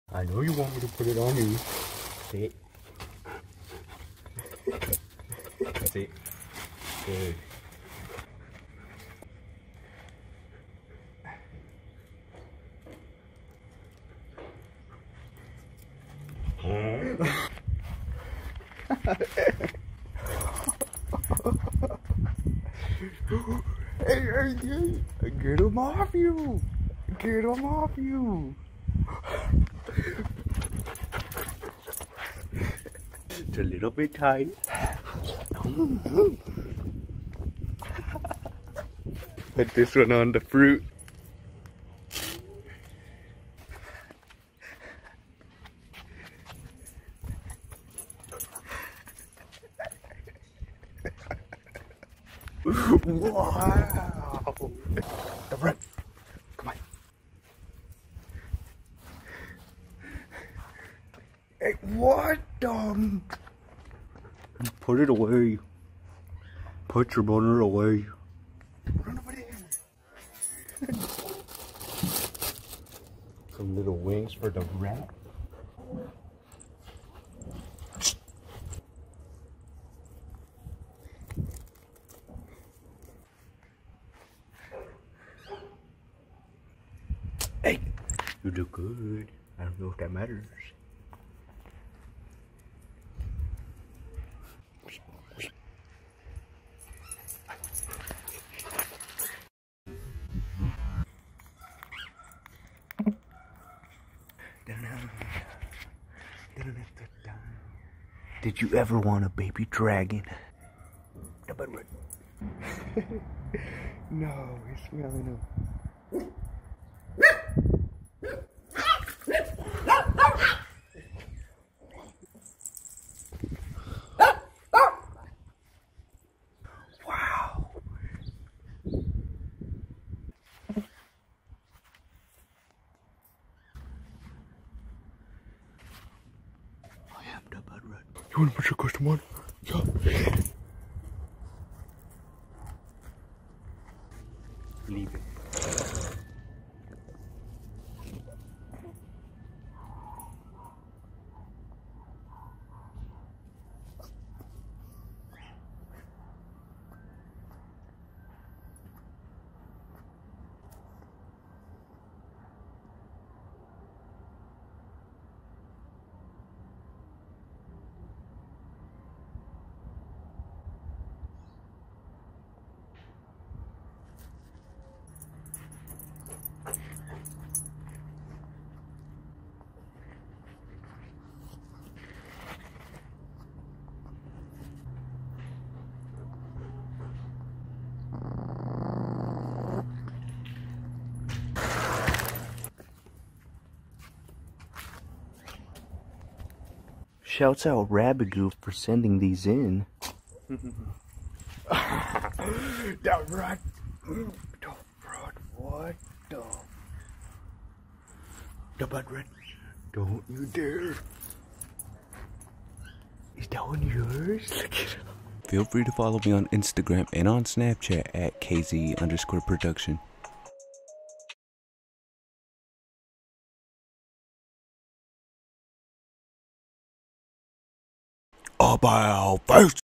My Dogs Get Jealous Of Each Others Halloween Costume *Vicious Bat Rat Attac Husky*
Chihuahua Da Rat Dressed As A Bat For Halloween. Jealous Dogs. Dog Halloween Costume Contest Vicious Barking Causes Puppy To Panic. Husky howling of excitement. howling crying talking training puppies

CHANNEL INFO